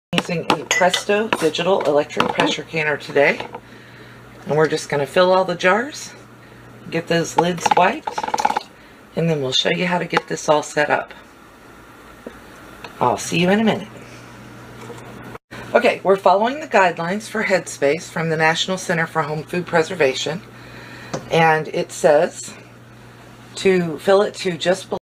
Canning Chili in an Electric Pressure Canner
In this video, we are canning chili in an electricpressurecooker